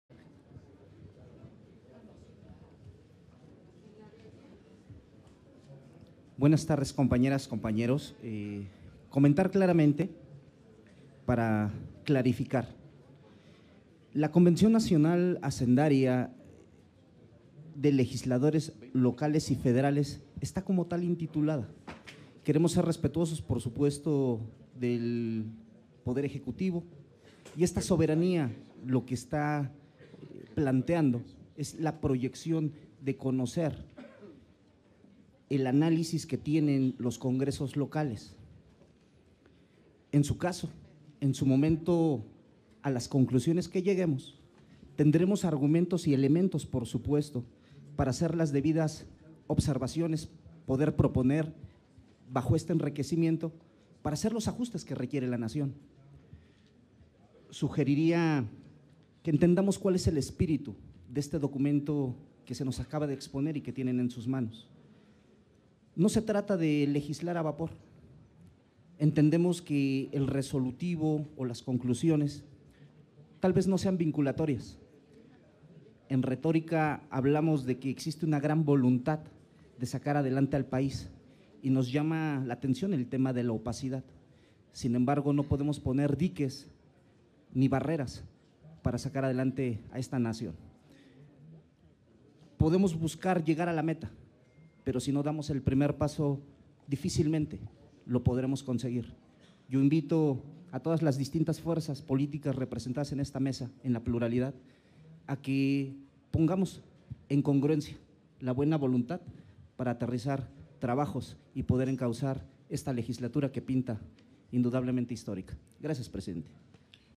30/10/2018 Participación del Dip. César Agustín Hernández Pérez
Participación del Dip. César Agustín Hernández Pérez en la reunión ordinaria de la comisión de presupuesto y cuenta pública